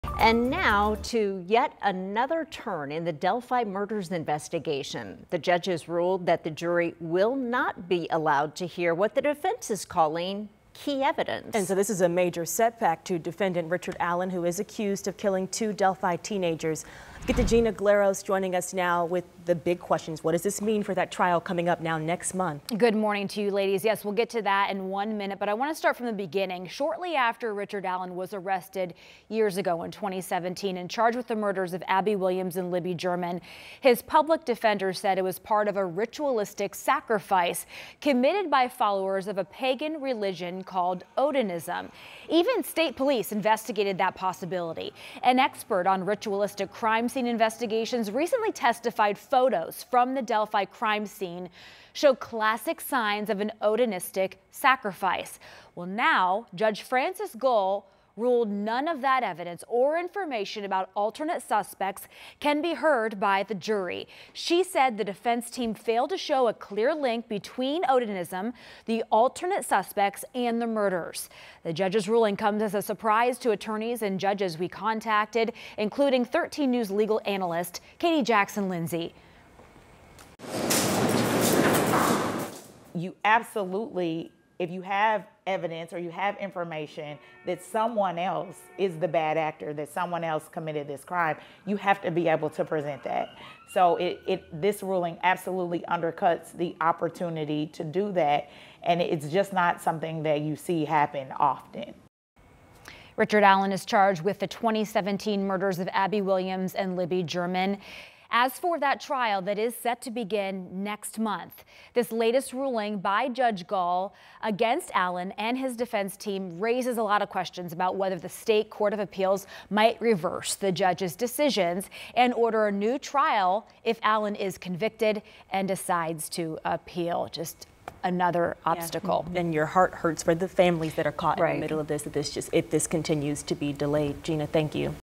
Judge: No Odinism, alternate suspects at Delphi murders trial
Special Judge Frances Gull granted the State's Motion in Limine blocking any mention of Odinism or the names of almost a dozen alternate suspects during the trial.